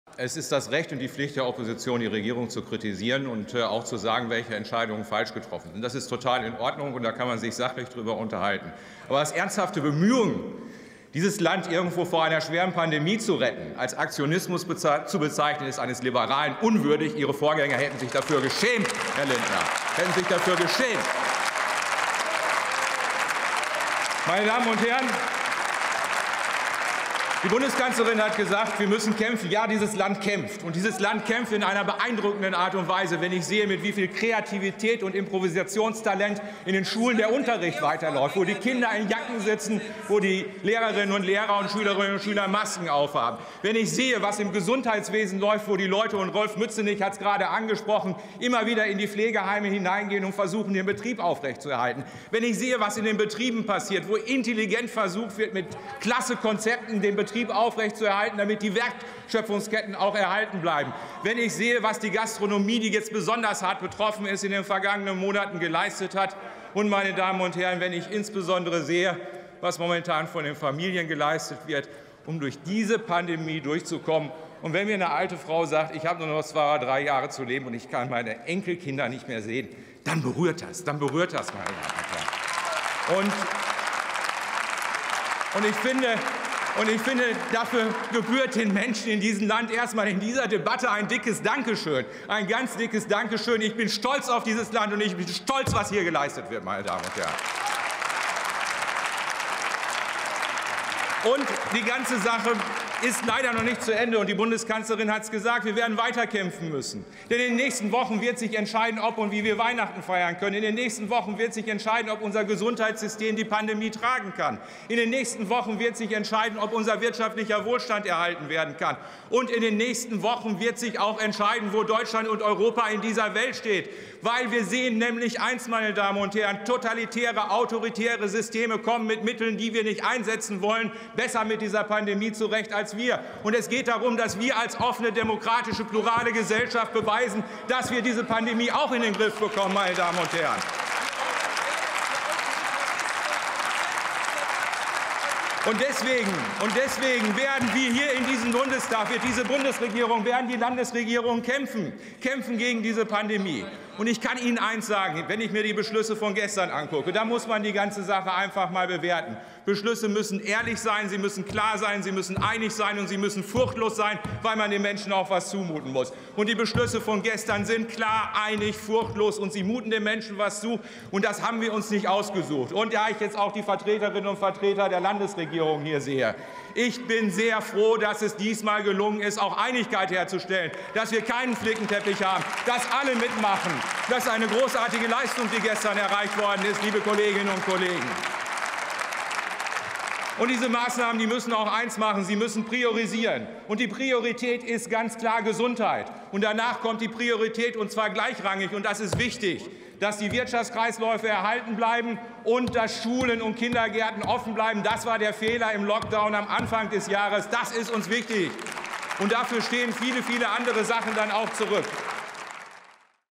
Emotionale Rede von Ralph Brinkhaus im Bundestag
Mein Kollege und Fraktionsvorsitzender der CDU/CSU-Bundestagsfraktion reagiert nach der Regierungserklärung der Kanzlerin auf die anschließende Kritik von Christian Lindner (FDP). Corona-Maßnahmen sind notwendig und und retten Leben. Von Aktionismus könne keine Rede sein - es brauche Zusammenhalt, statt Spaltung. Unbedingt anschauen!

Der CDU-Bundestagsabgeordnete Andreas Steier berichtet über seine politische Arbeit in Berlin für seine Heimat Trier und Trier-Saarburg.